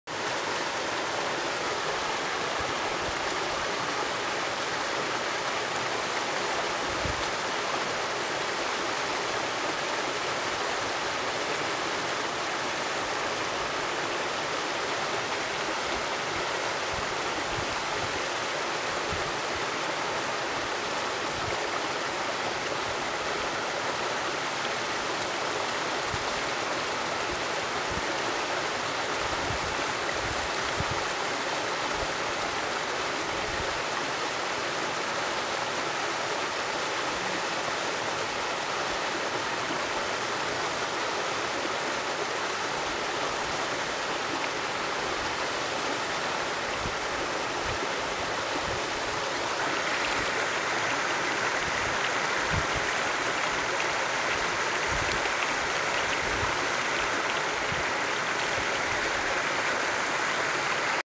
Tres pozas- Mexicali Baja California, Cañon del diablo.
Belleza increible de nuestro estado, cañon que permanece en su estado virgen ubicado en Mexicali, Baja California.